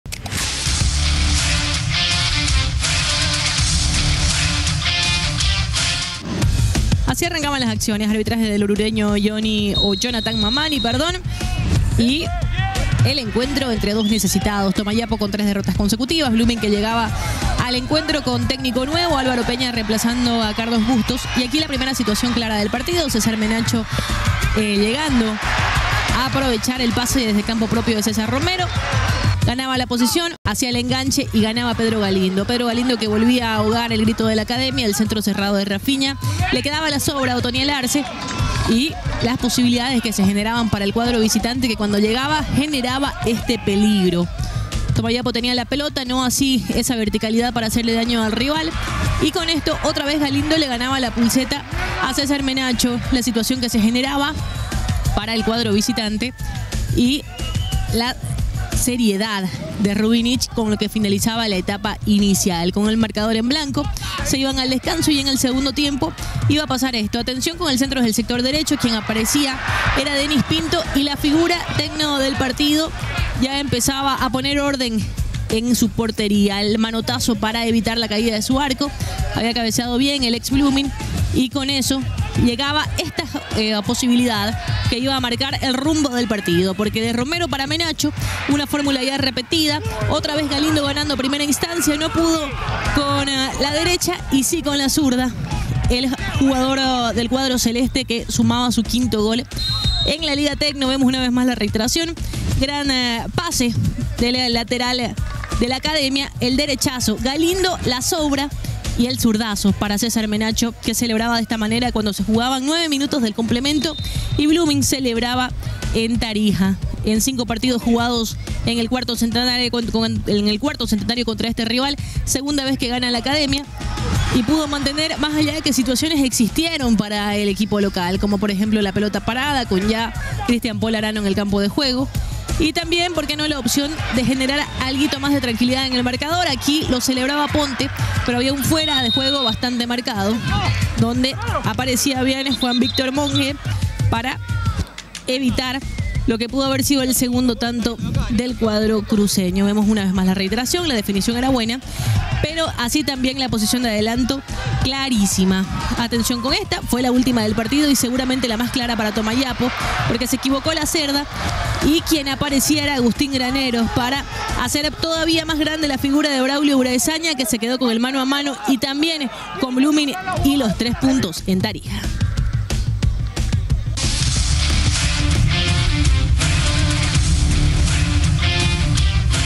¡DEBUT CON VICTORIA PARA PEÑA! Blooming se impuso a Tomayapo en Tarija
📲Android 📲 Iphone